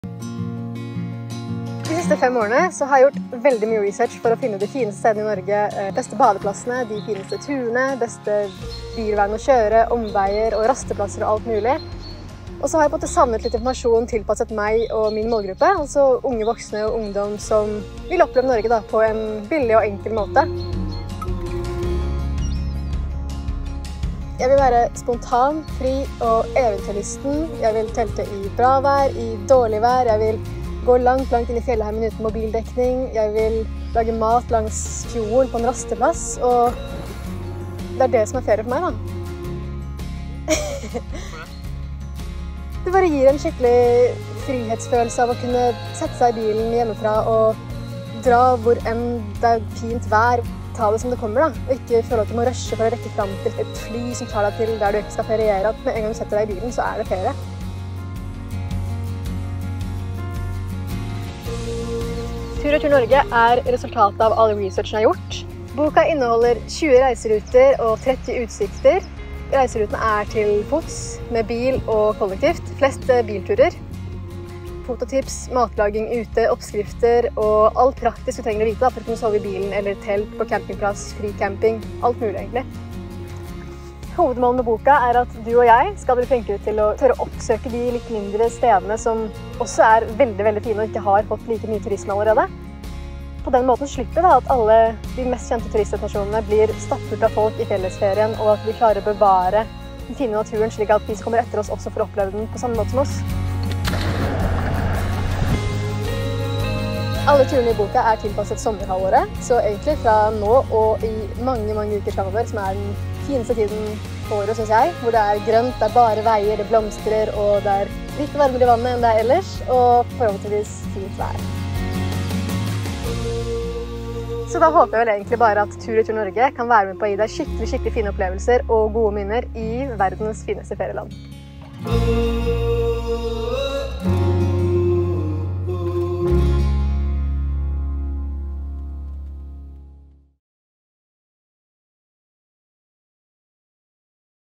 Promovideo for boken Tur-retur Norge med
Årets flotteste turbok fra skaperen av den populære instagramkontoen @helenemoo!

Helene er 23 år og har to hovedinteresser i livet: foto og friluftsliv. I denne boken har hun samlet alt det beste fra instagramkontoen: turforslag til både korte og lange turer i Norge både med bil og til fots, handlelister, pakkelister, turmat, kart, notatsider og mye annet den turglade vil få bruk for. I tillegg deler Helene sine beste tips for hvordan man tar de beste bildene underveis.